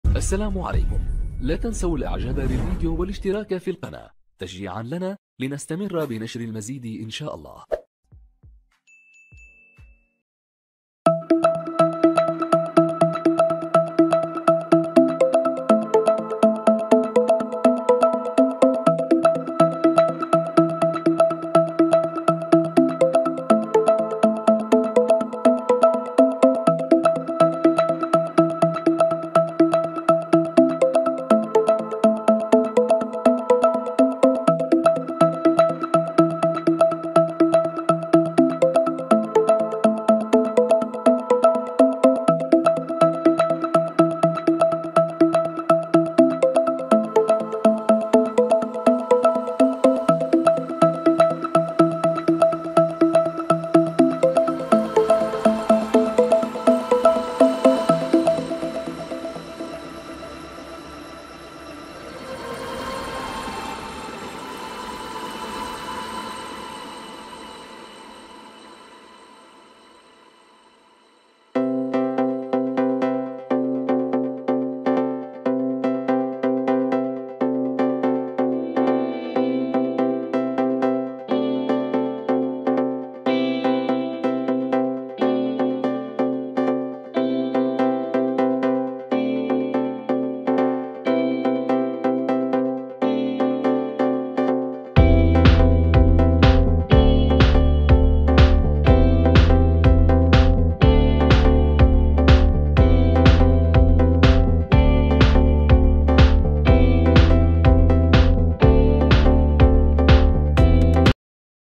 machine a pop corn electrique آلة الفشار الكهربائية
هذا الفيديو اعددته عن موضوع : Ce produit est vendu par société prima cuisine dreb Omar Casablanca